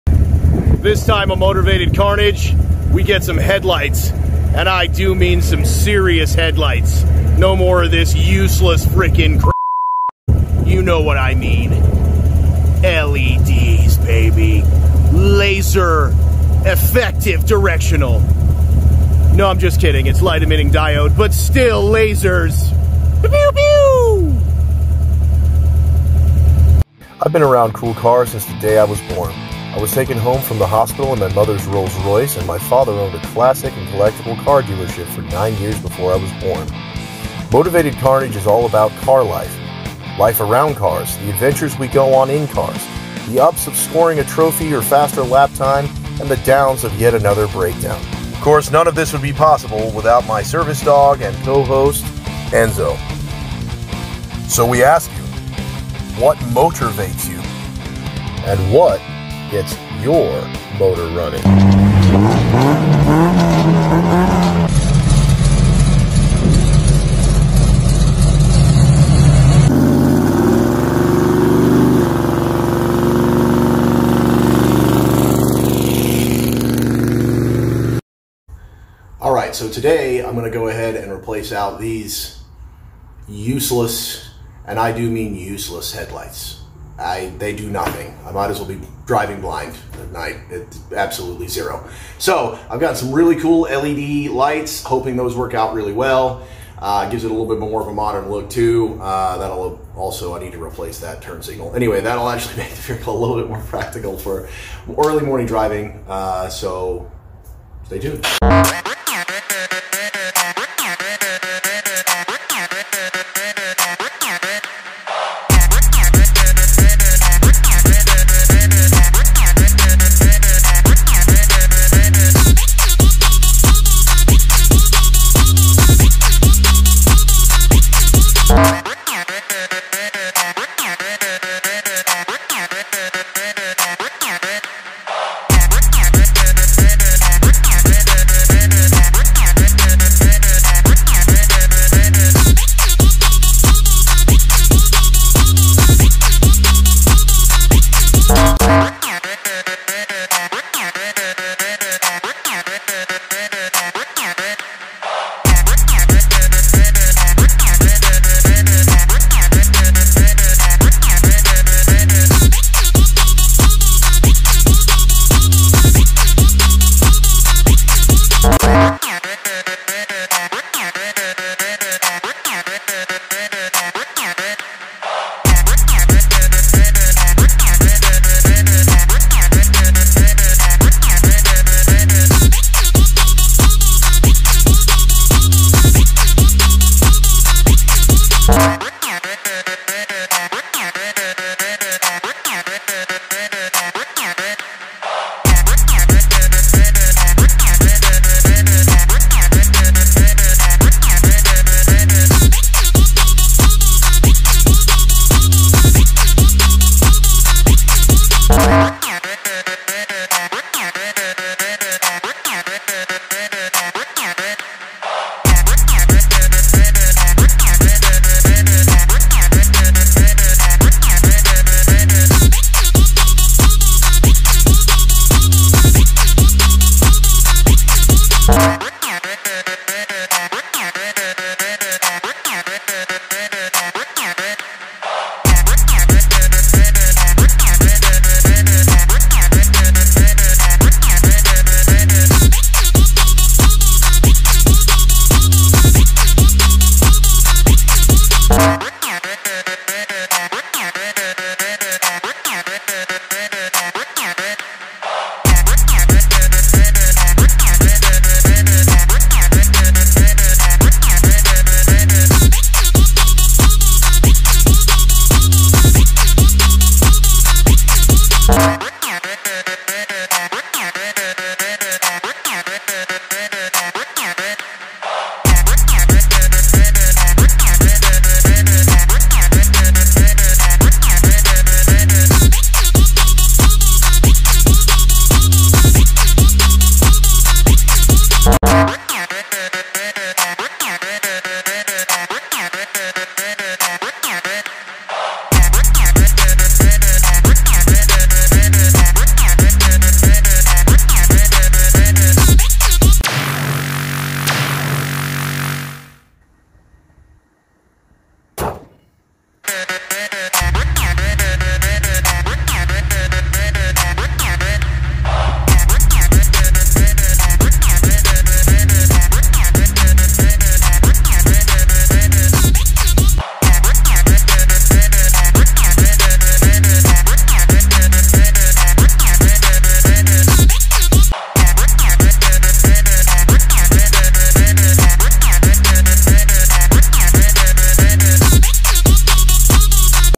The Grape Ape gets head....lights or lasers you be the judge!!
The Grape Ape gets headlights and a restyle/restoration on its front end. I think it gave the truck a much more aggressive style but what do you think??